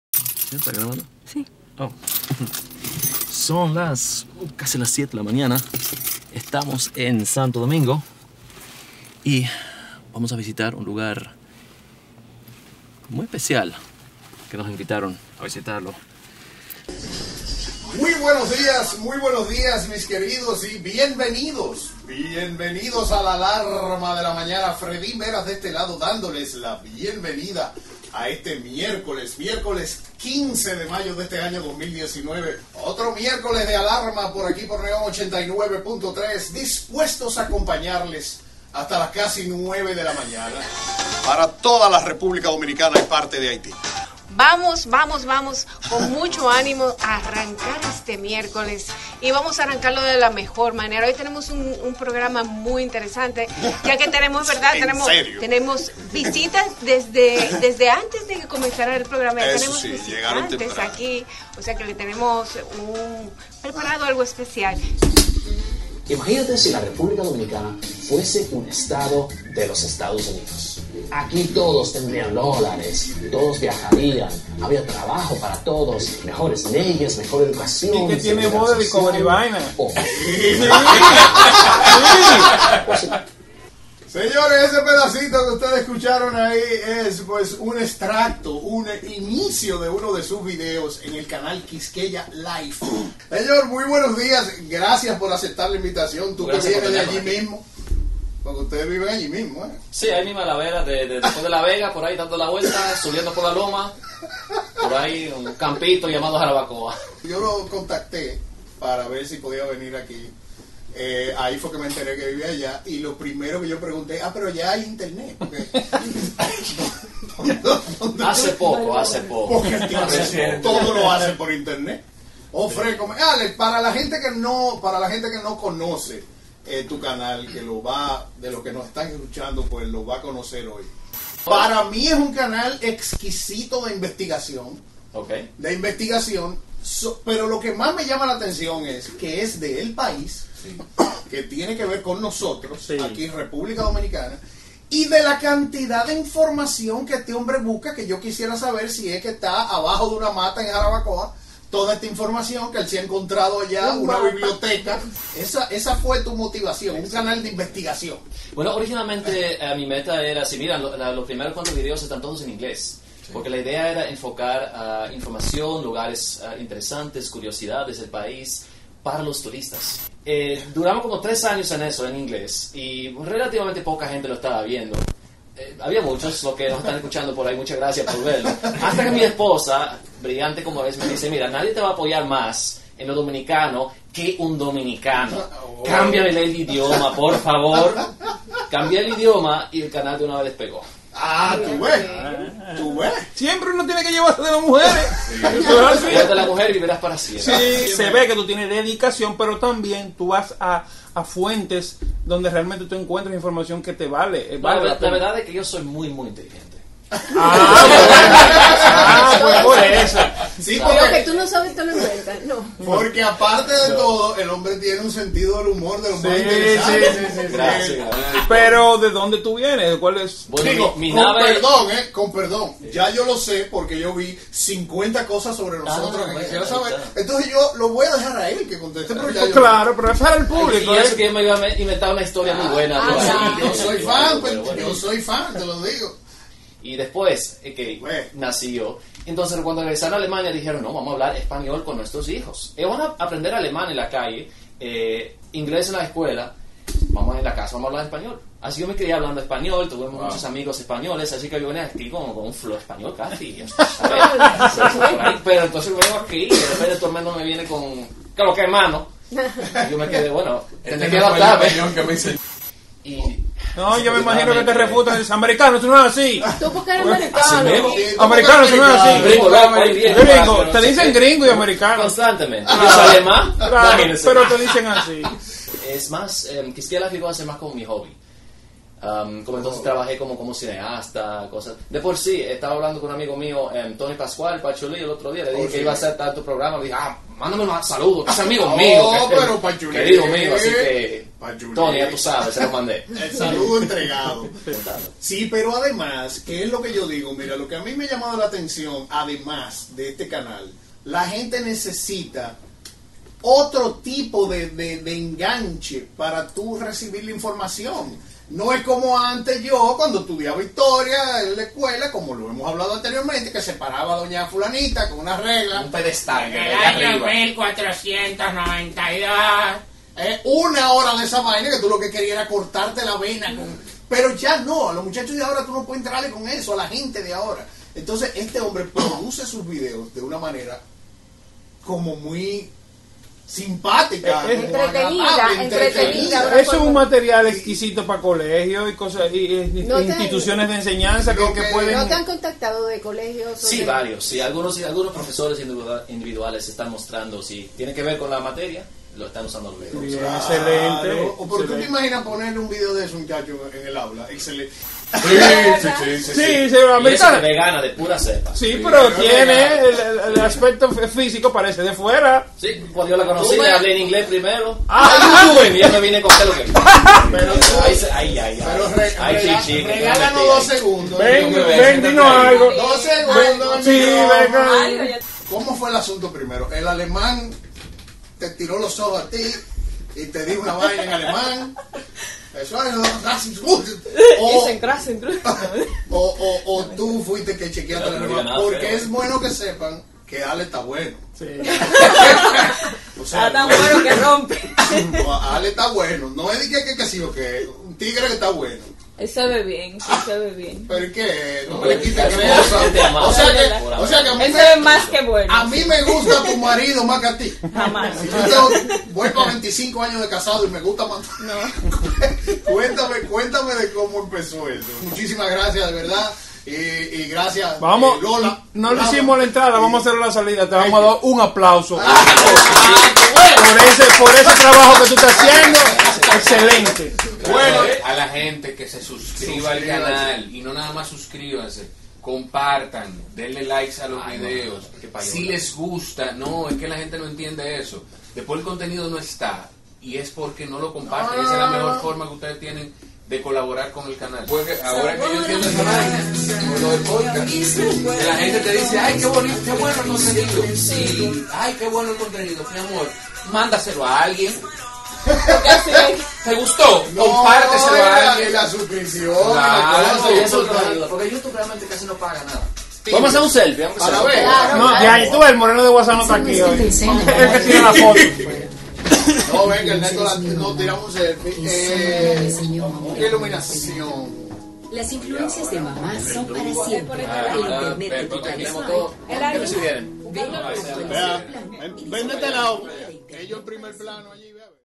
Freddyn Beras-Goico entrevista KiskeyaLife (video blog)
Tuvimos el privilegio de estar en el programa de Freddyn Beras-Goico hablando de Kiskeya Life!

Visite para mas vídeos y artículos en Inglés y Español sobre Quisqueya!

¡Duda, investiga y aprende!

Las cámaras y nuestro equipo: